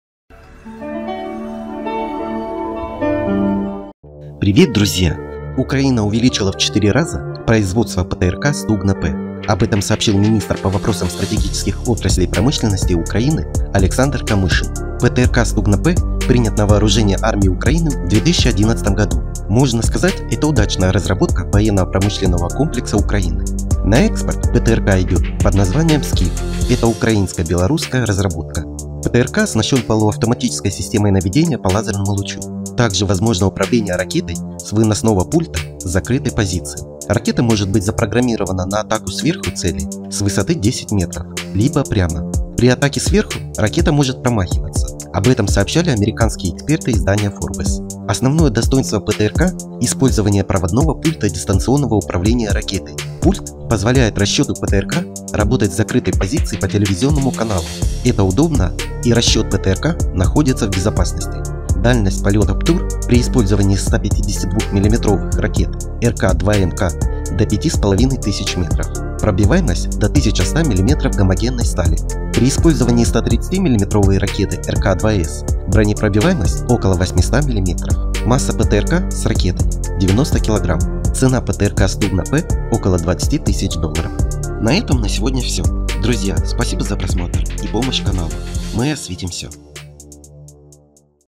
Выпуск ПТРК Стугна-П увеличен в Украине, обзор
Украина увеличила в 4 раза производство ПТРК "Стугна-П", об этом сообщил министр по вопросам стратегических отраслей промышленности Украины Александр Камышин. ПТРК «Стугна-П» принят на вооружение армии Украины в 2011 году. Можно сказать это удачная разработка военного промышленного комплекса Украины. На экспорт ПТРК идет под названием Skif, это украинско-белорусская разработка. ПТРК оснащён полуавтоматической системой наведения по лазерному лучу. Также возможно управление ракетой с выносного пульта с закрытой позиции. Ракета может быть запрограммирована на атаку сверху цели, с высоты 10 метров, либо прямо. При атаке сверху ракета может промахиваться, об этом сообщали американские эксперты издания Forbes. Основное достоинство ПТРК, использование проводного пульта дистанционного управления ракетой. Пульт позволяет расчету ПТРК работать с закрытой позиции по телевизионному каналу, это удобно и расчет ПТРК находится в безопасности. Дальность полета ПТУР, при использовании 152 мм ракет РК-2М-К до 5500 метров, пробиваемость до 1100 мм гомогенной стали. При использовании 130 мм ракеты РК-2С, бронепробиваемость около 800 мм. Масса ПТРК с ракетой – 90 кг. Цена ПТРК «Стугна-П» около 20 тысяч долларов.